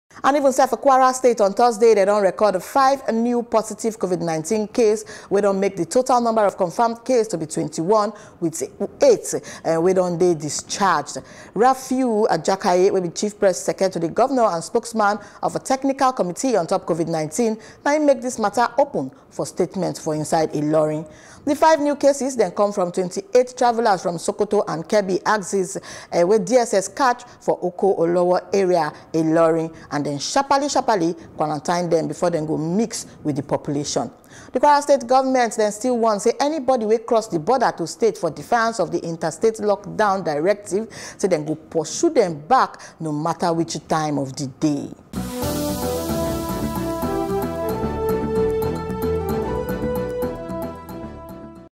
Kwara Records 5 New Covid 19 Cases
Kwara State on Thursday don record five new positive COVID-19 cases, wey don the total number of confirmed cases to be 21 with eight wey son dey discharged.